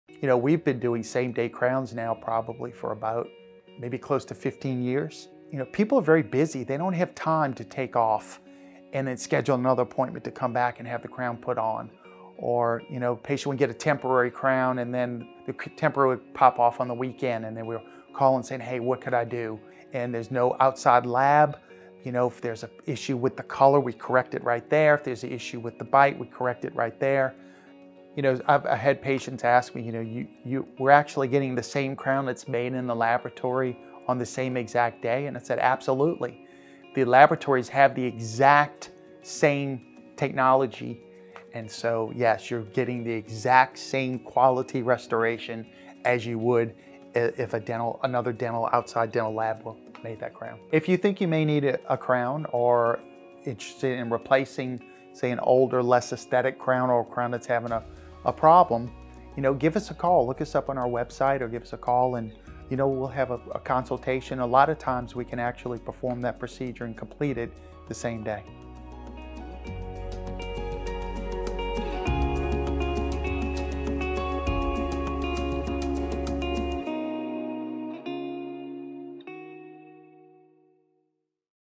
Service Video | Mandeville Center For Dental Excellence (2nd Shoot) | Mandeville, LA - SameDay Crown
Dr. Charles Schof of Mandeville Center For Dental Excellence was so happy with the video production shoot and the dental practice branding video we did for him in 2019, he had us come back to do more videos!